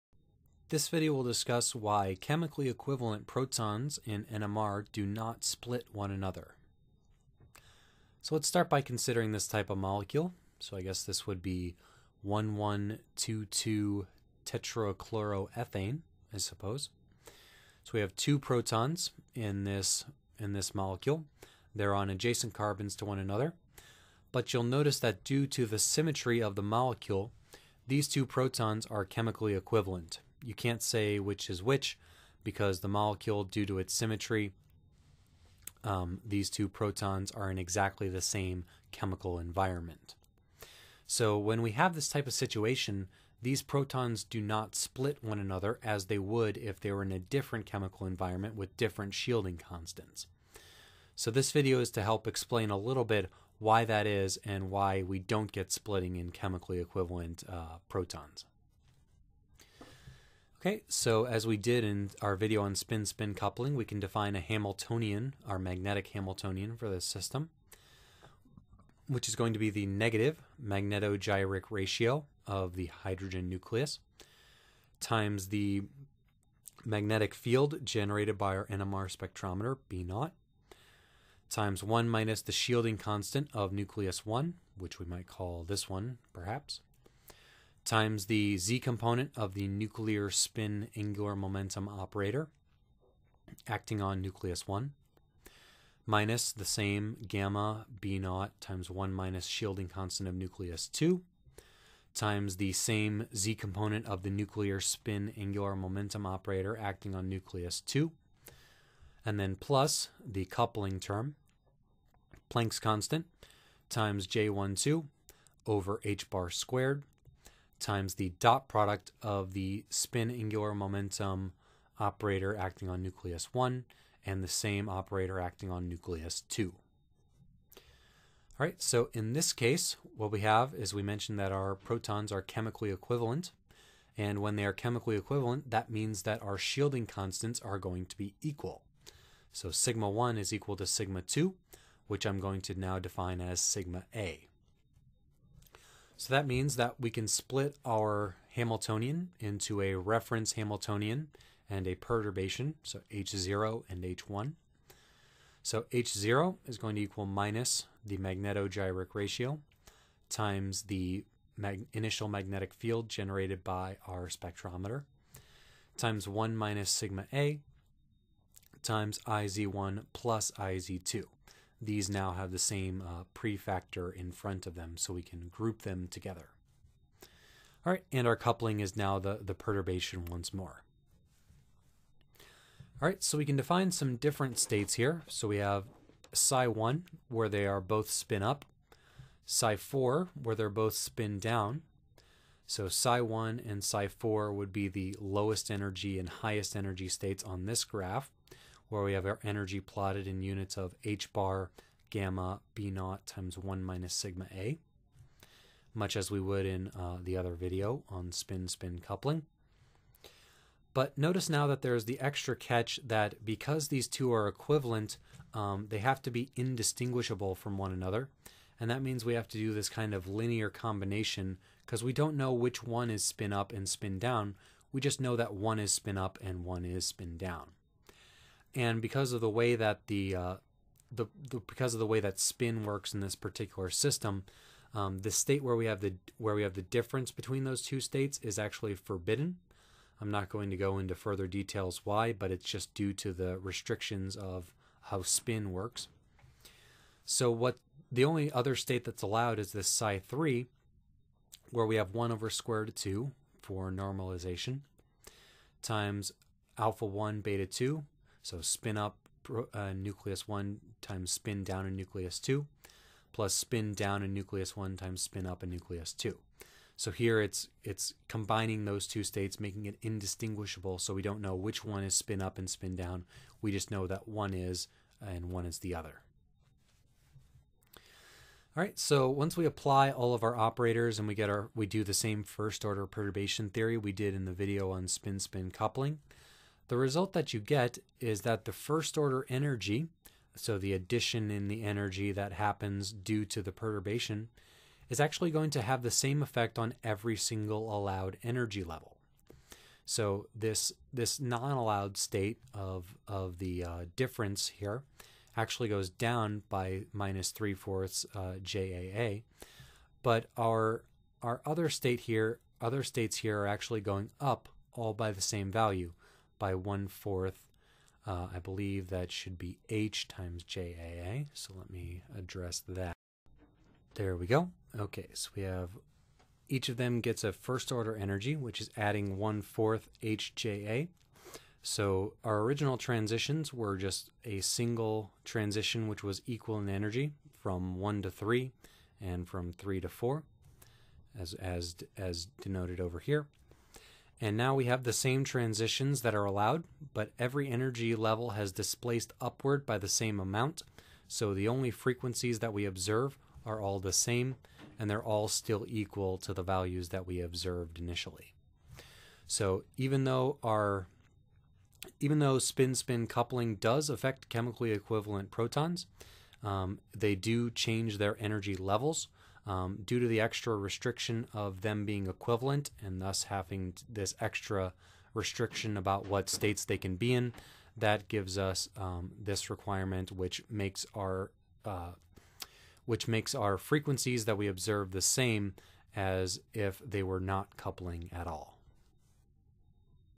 Quantum Chemistry 13.9 - Chemical Equivalence
Short lecture on chemical equivalence in NMR.

Nuclei which are chemically equivalent (i.e. the same magnetic shielding constant) that couple their nuclear spins results in an equal increase in all energy levels, leaving the NMR spectrum unaffected, and the magnetic resonance peaks non-split.

--- Video Links ---

--- Social Links ---

--- Equipment ---

Drawing Tablet: Wacom Intuos Pen and Touch Small

Drawing Program: Autodesk Sketchbook Express